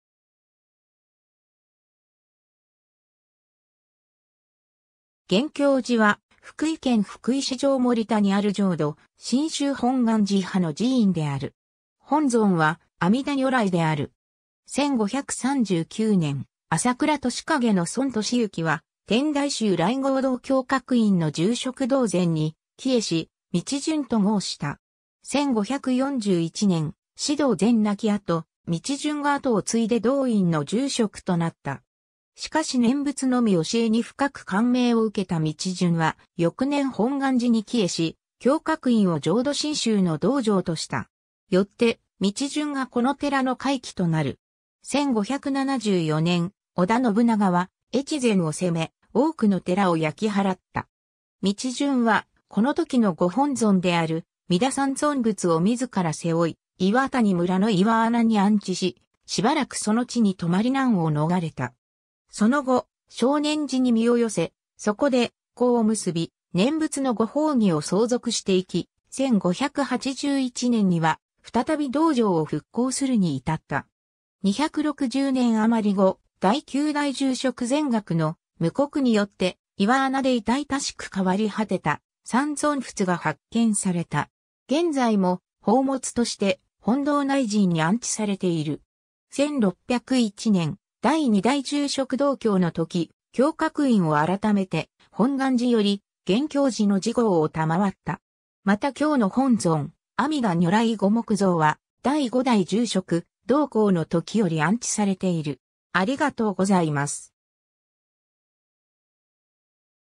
厳教寺
厳教寺（ごんきょうじ）は、福井県福井市上森田にある浄土真宗本願寺派の寺院である。
本尊は阿弥陀如来である。
 1539年（天文8年）、朝倉敏景の孫敏行は、天台宗来迎堂教覚院の住職道全に帰依し、「道順」と号した。
1541年、師道全亡き後、道順が後を継いで同院の住職となった。
しかし念仏のみ教えに深く感銘を受けた道順は、翌年本願寺に帰依し、教覚院を浄土真宗の道場とした。
よって道順がこの寺の開基となる。
 1574年（天正2年）、織田信長は越前を攻め、多くの寺を焼き払った。
道順は、この時のご本尊である弥陀三尊仏（阿弥陀如来、観音菩薩、勢至菩薩）を自ら背負い、岩谷村（丸岡町山竹田）の岩穴に安置し、暫くその地に留まり難を逃れた。
 その後、称念寺（丸岡町長崎）に身を寄せ、そこで「講」を結び、念仏のご法義を相続して行き、1581年(天正9年)には、再び道場を復興するに至った。
 260年余り後、第9代住職善学の「夢告」によって、岩穴で痛々しく変わり果てた「三尊仏」が発見された。
現在も宝物として、本堂内陣に安置されている。
 1601年（慶長5年）、第2代住職道興の時、教覚院を改めて、本願寺より「厳教寺」の寺号を賜った。
 また今日の本尊「阿弥陀如来御木像」は、第5代住職道光の時より安置されている。